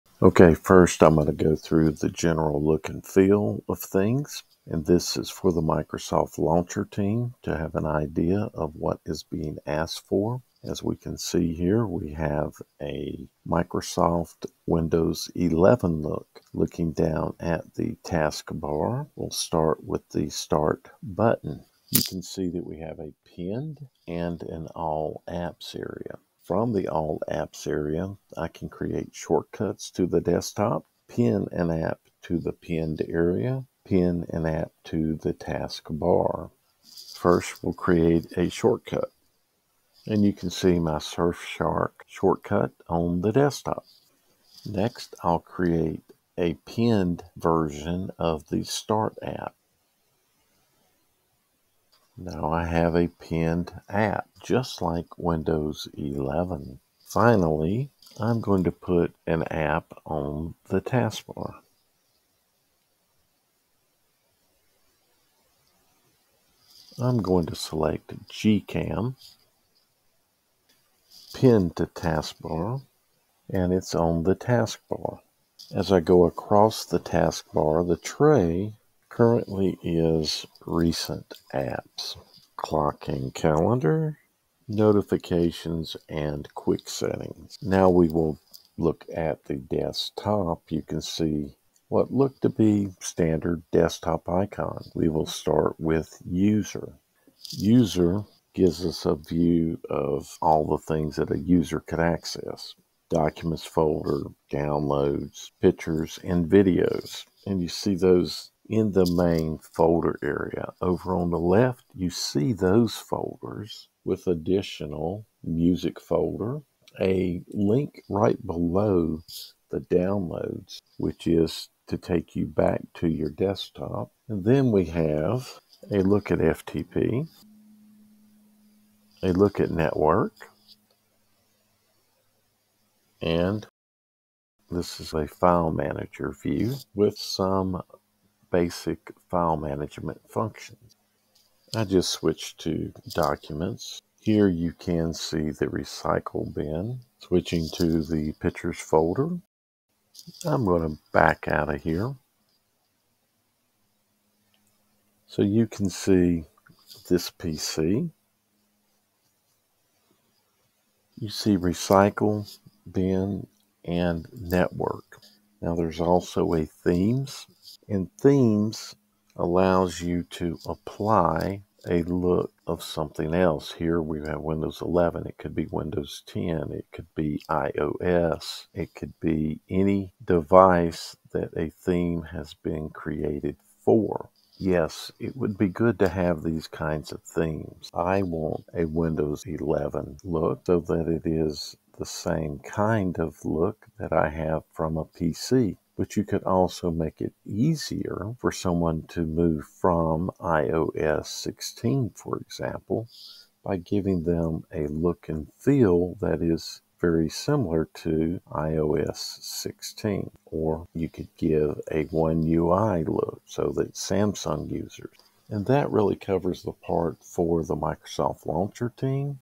Windows 11 Launcher Interface Demo on Surface Duo for Microsoft Launcher Team
This is a demonstration of a Windows 11 Launcher Interface on Microsoft Surface Duo